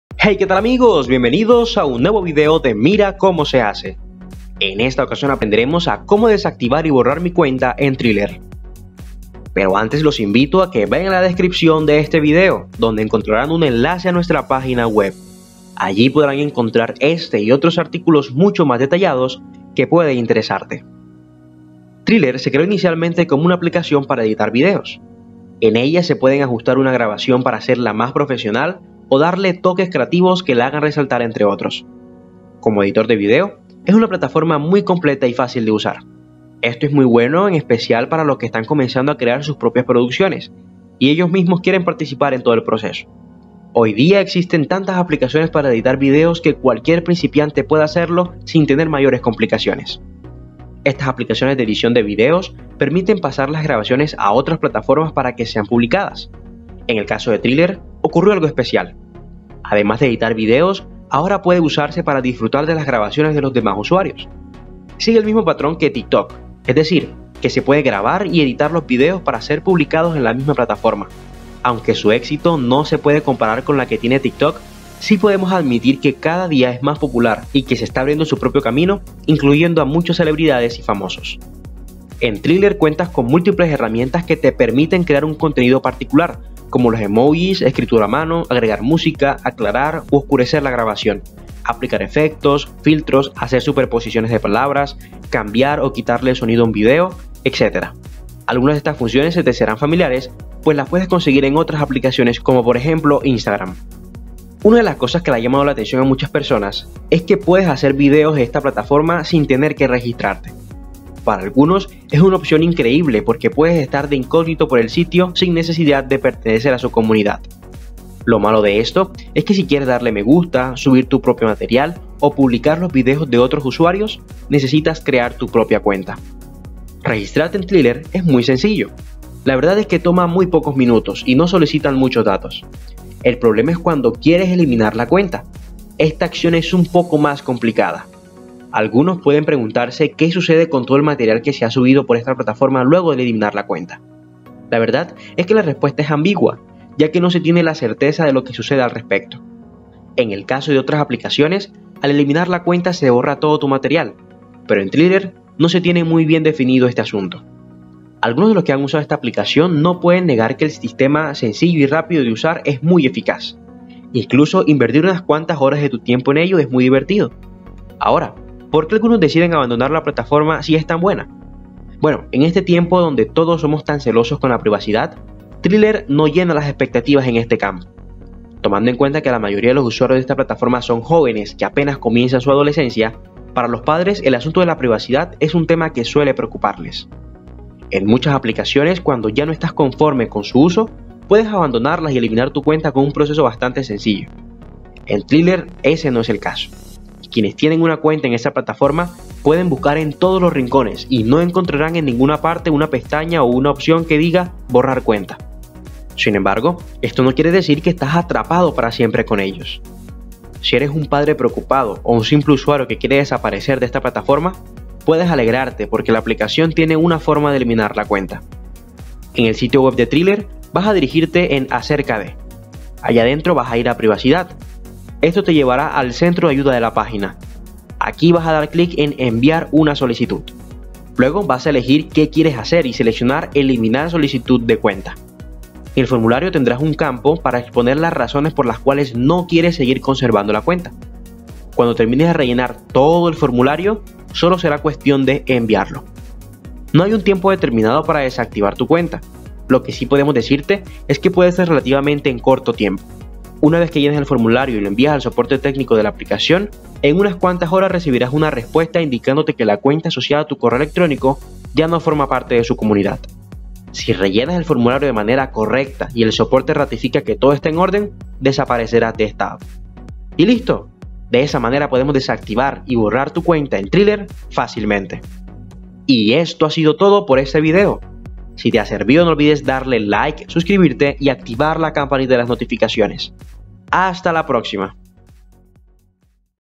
¿Cómo Desactivar y Borrar mi Cuenta en Triller? - Fácil y Sencillo
En este video te mostramos Cómo Desactivar y Borrar mi Cuenta en Triller. En este canal encontrarás los mejores tutoriales. ¡No te lo pierdas!